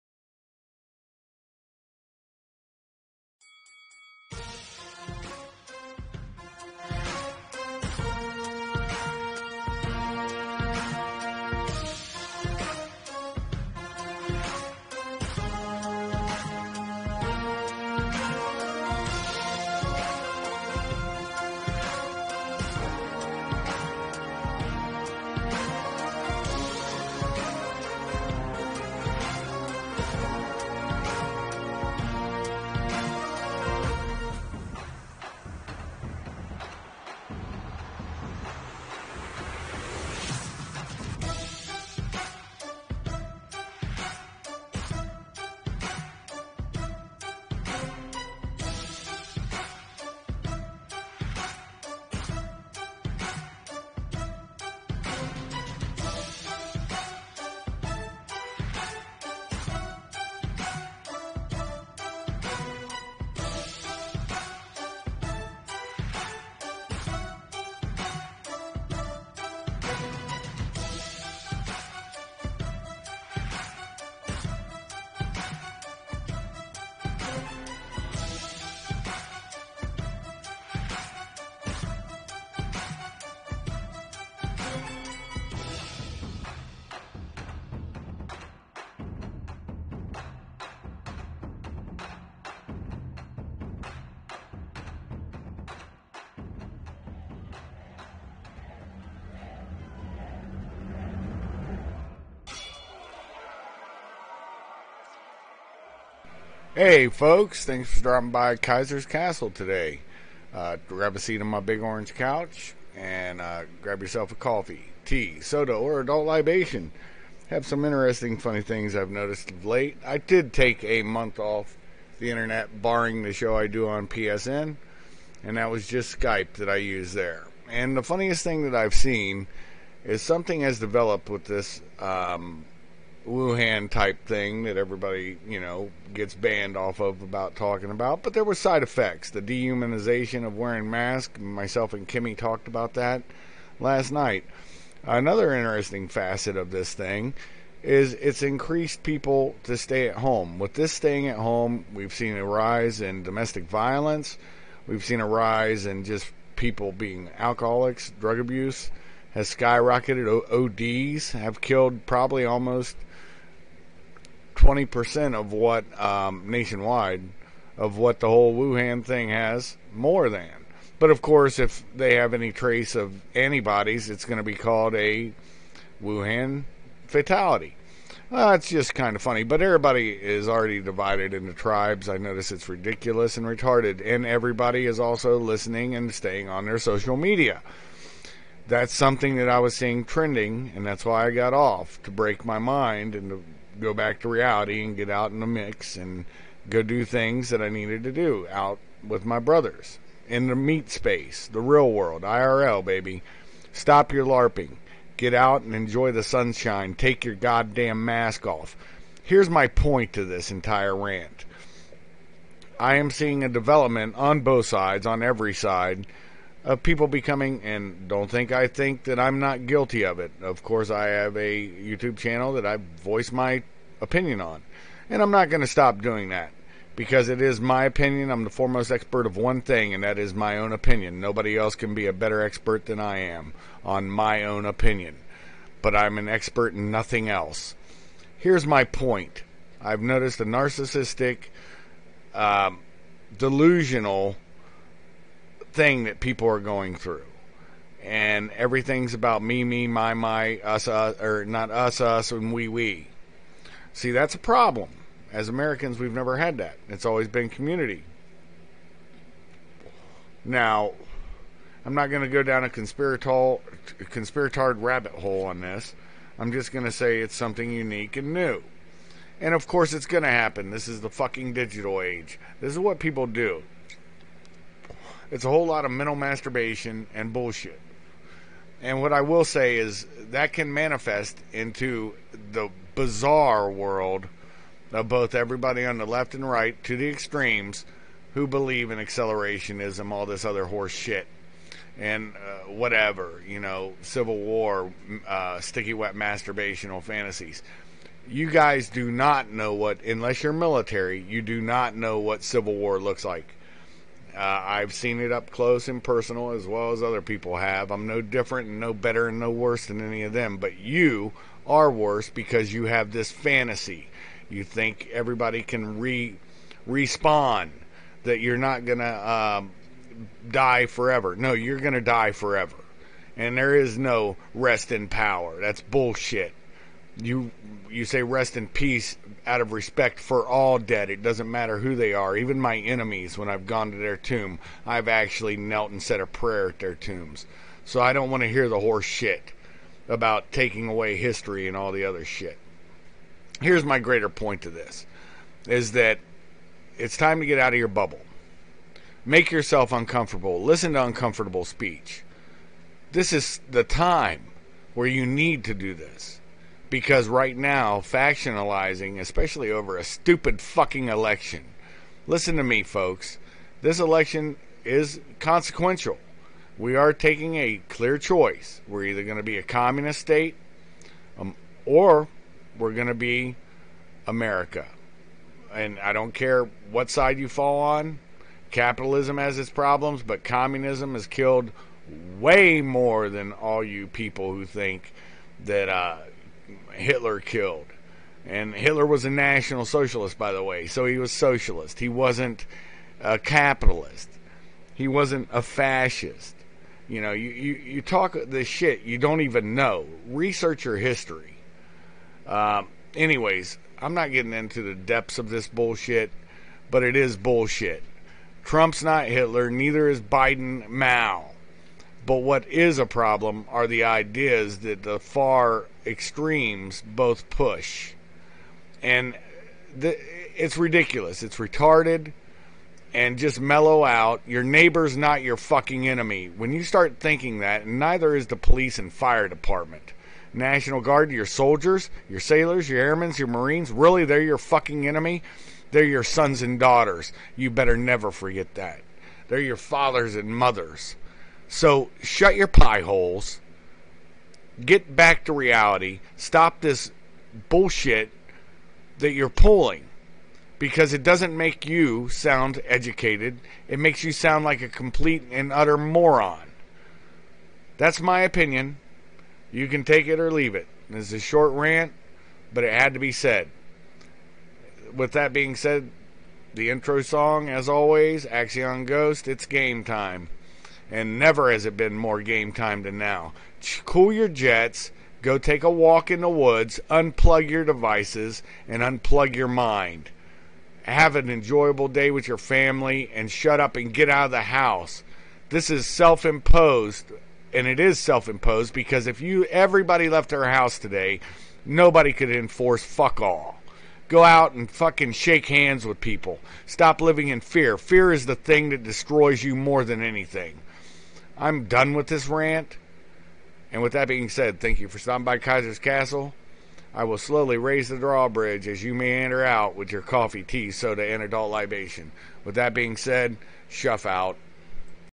My Rant from Kaisers Kastle. Break out of your bubbles and echo chambers!
Get out into the Meat Space and talk to your neighbors. They are not your enemies! Fear is your Enemy...